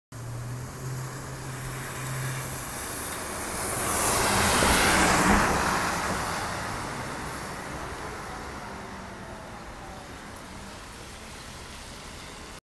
2015 New Flyer XD40 #7310 on the B46 Select Bus Service at Malcolm X Blvd and Decatur Street
Taken 5/31/18.

These 2015 XD40s are being pushed off of SBS service by the 2018 XD40s, and these will go back to local service.